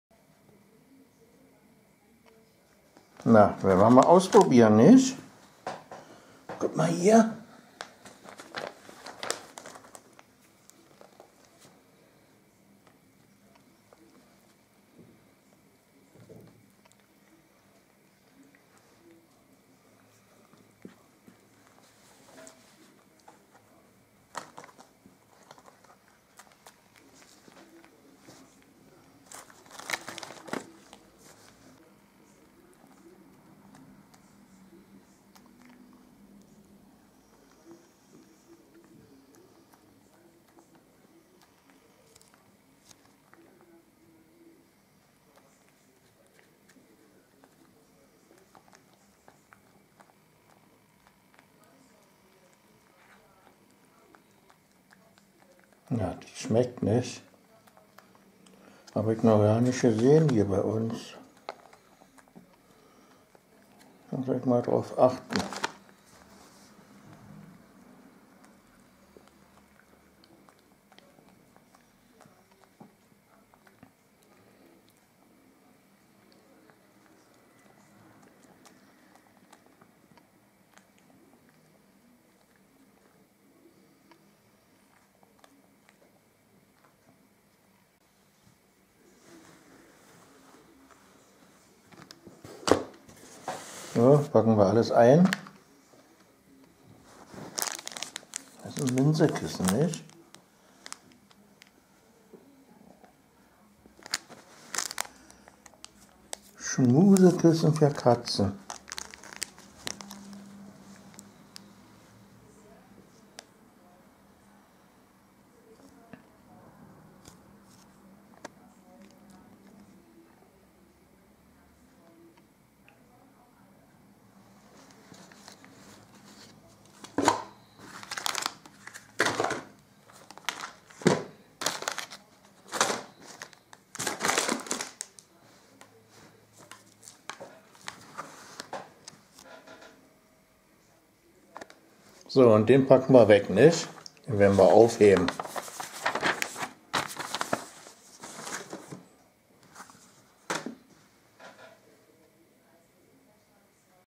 Püppi beschnuppert ihre Geschenke
Püppi Beschnuppert und probiert ihre Geschenke